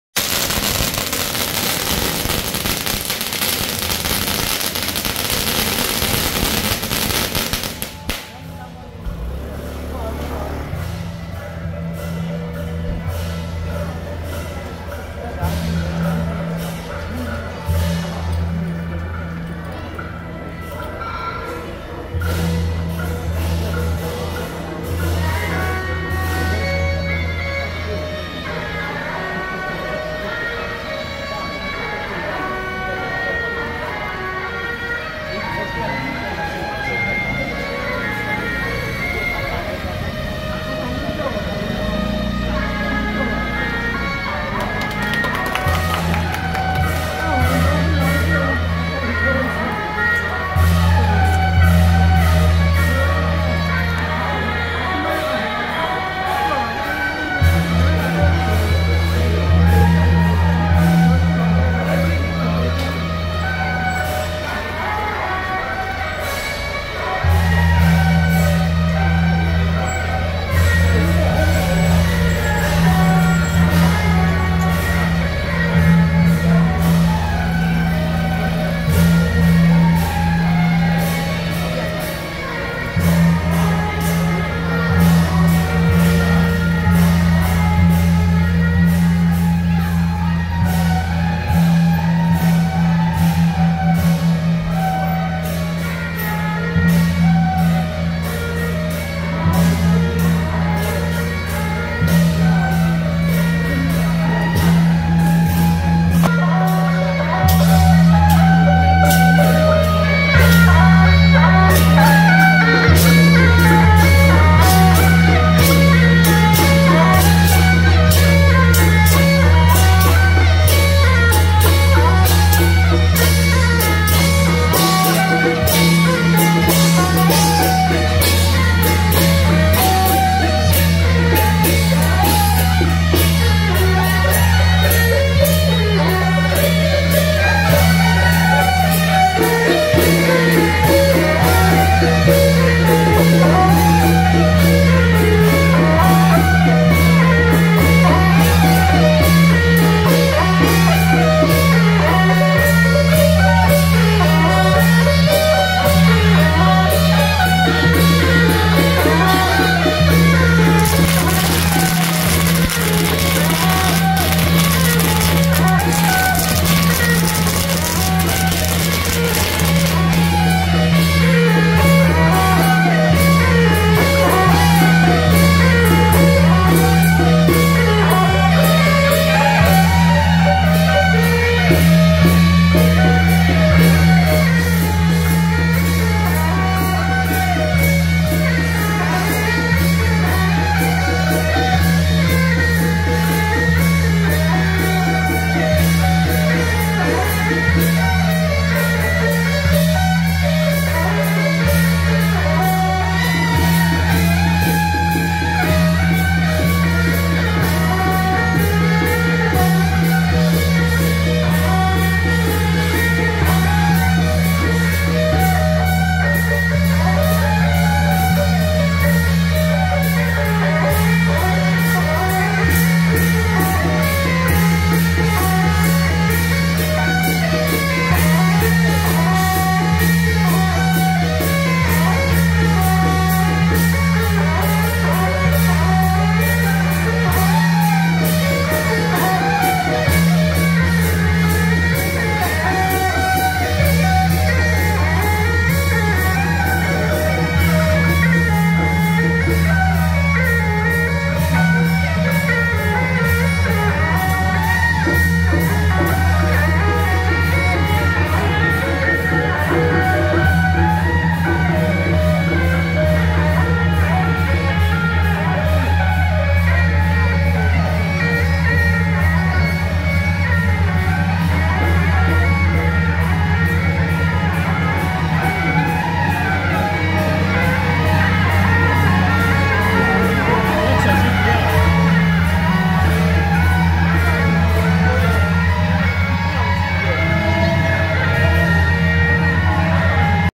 【新莊新樂園 慶贊 中和清和社】~112中和清和社創社百年大囍暨恭祝西秦王爺聖誕出巡遶境
新莊新樂園-執事人員,頭旗西燈車,三角旗車,彩牌車,北管＆神將團